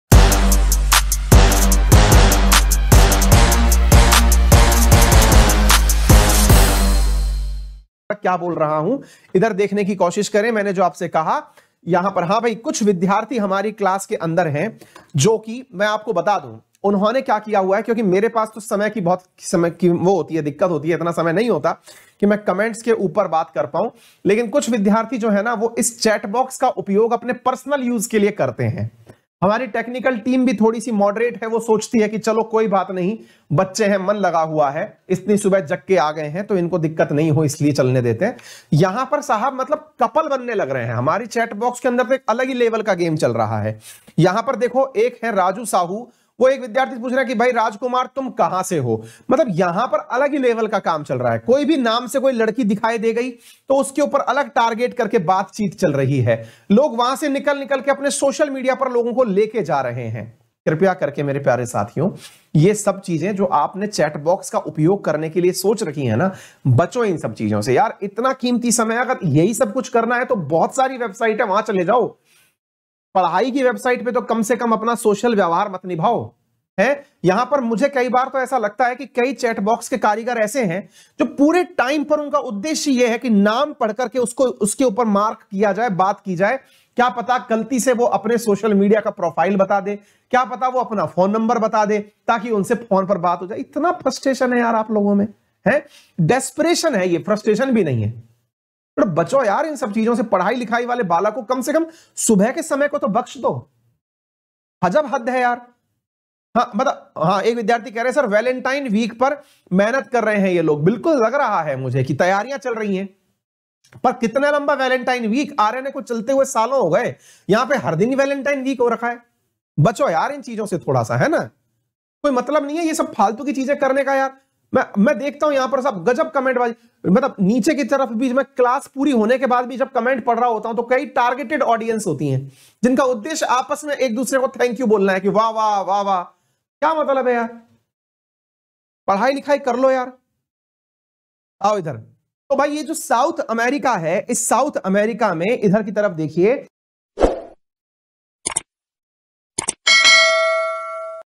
देखिए अंकित अवस्थी SIR ने कैसे LIVE SESSION में गंदा COMMENT करने वालों की GAND मार ली/Xstream News
Please like all my videos

This video only for enterteinment to kripa karke comment me entertainment na kare
Thank you for wachting

credit --- = wifi study
 from Ankit Sir